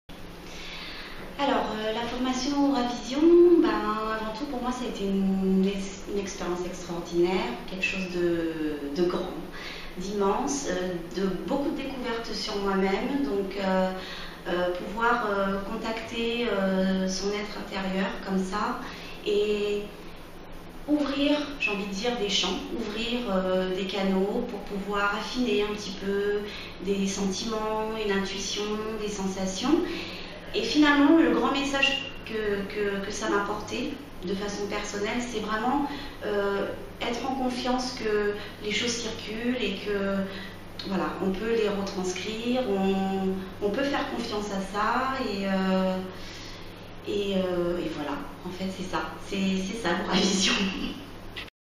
Témoignage aura vision Bordeaux
Témoignage de participant(e)s à la formation Aura Vision animée par Isabelle David